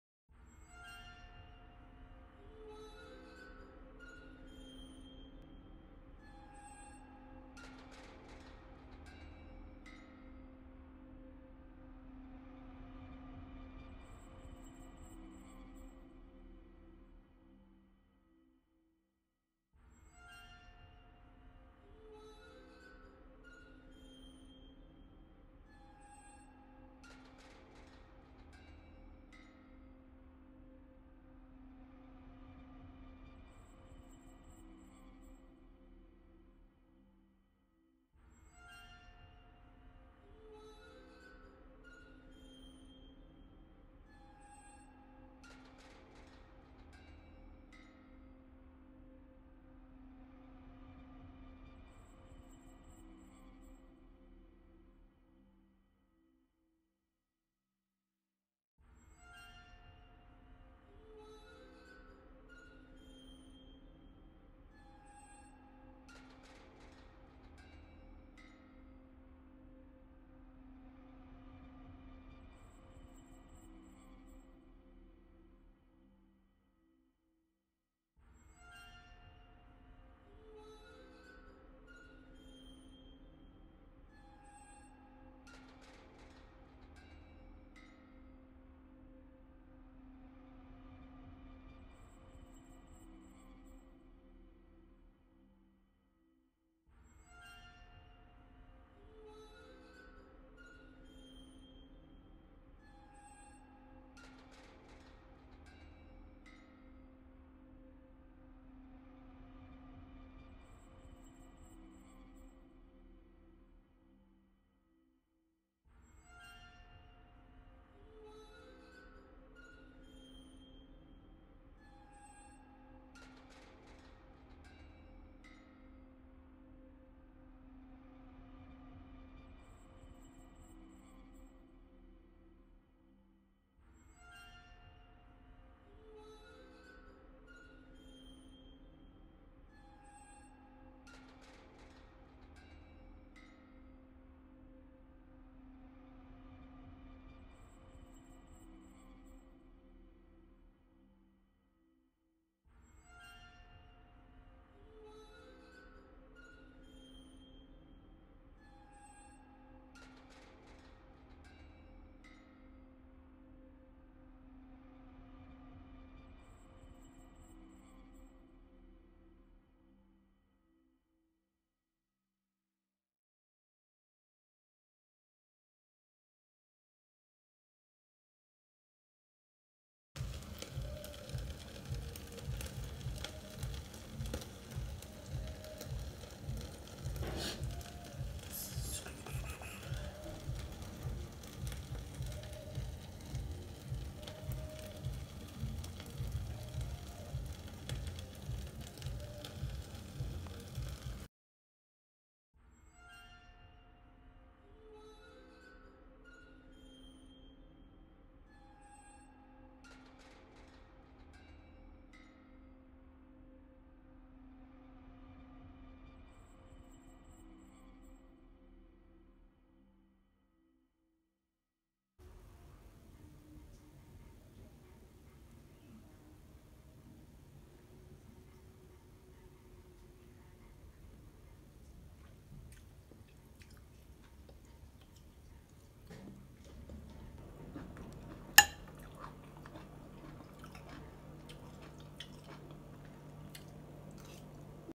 How To Prepare Spicy Gizzards Sauce In 4Minutes
Hi guys! Welcome to Yarh Yarh Tv.
Today we are preparing one of my favorite dishes, gizzards sauce. So guys stick and stay as I take you through this recipe

Ingredients:
Gizzards
Garlic
Onion
Scallions
Carrots
Habanero pepper
Green pepper
Spices
Vegetable oil